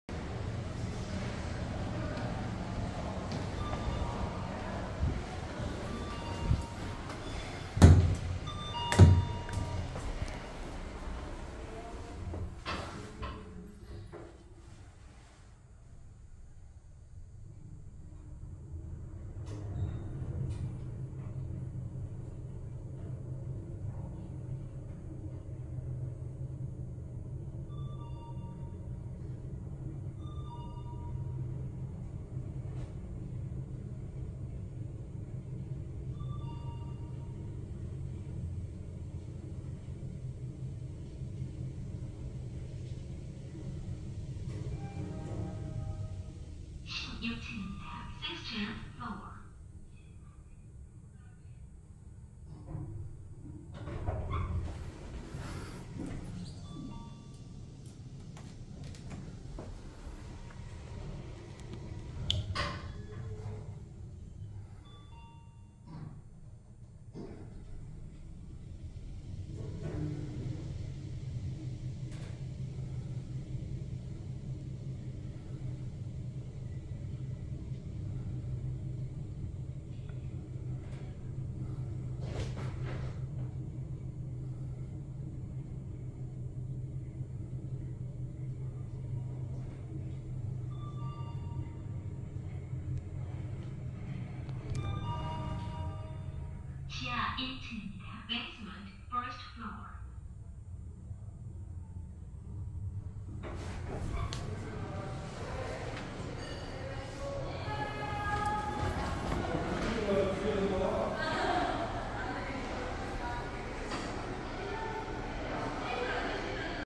서울특별시 강남구 테헤란로 152 (역삼동) 강남파이낸스센터 나머지구간 6 현대엘리베이터 리모델링전 탑사기
원활한 탑사진행을 위해 B1층에서 시작했으며, 6월초에 OTIS엘리베이터로 리모델링할 예정이니 서둘러서 탑사하시기 바랍니다.

* 여기는 1층에 보안요원이 있거나 상당히 혼잡할 수 있으니 탑사시 주의하십시오.